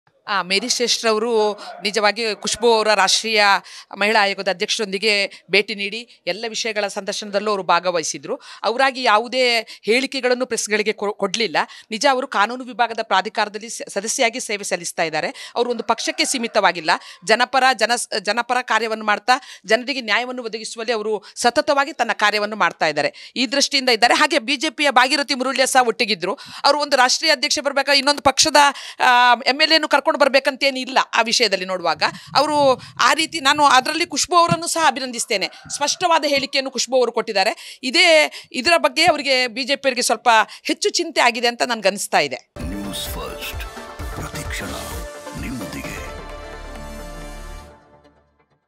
Sunitha Shetty : BJPಯವ್ರಿಗೆ Kushboo ಹೇಳಿಕೆದೇ ಚಿಂತೆ ಆಗಿದೆ.. | Udupi Washroom Incident |Newsfirst
Sunitha Shetty : BJPಯವ್ರಿಗೆ Kushboo ಹೇಳಿಕೆದೇ ಚಿಂತೆ ಆಗಿದೆ.. | Udupi Washroom Incident | @newsfirstkannada